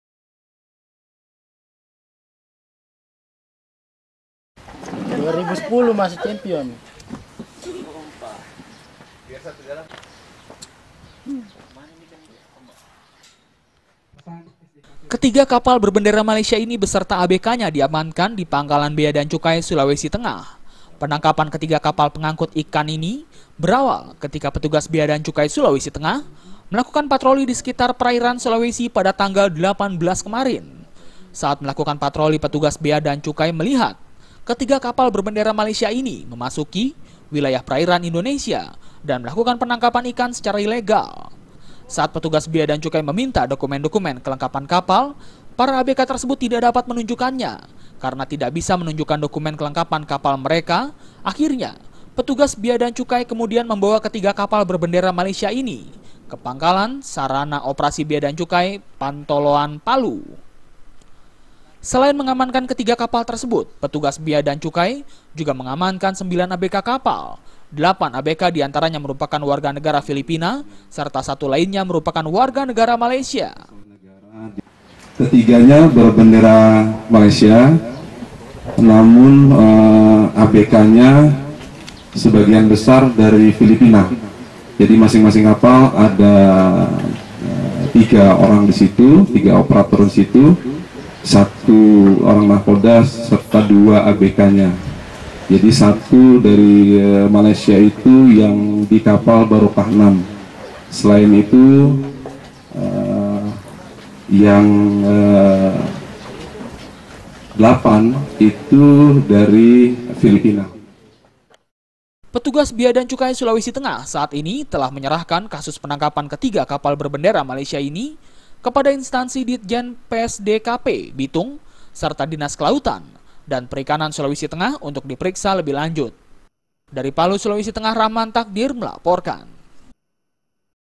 TIGA KAPAL MALAYSIA DITANGKAP
TIM DARI BEA DAN CUKAI SULAWESI TENGAH , MENANGKAP 3 KAPAL BERBENDERA MALAYSIA  DIWILAYAH PERAIRAN LAUT SULAWESI TENGAH , YANG  DIDUGA MELAKUKAN PENANGKAPAN IKAN SECARA ILEGAL ATAU ILEGAL FISHING . KETIGA KAPAL BESERTA ABKNYA , LANGSUNG DIAMANKAN KE PANGKALAN BEA DAN CUKAI SULAWESI TENGAH.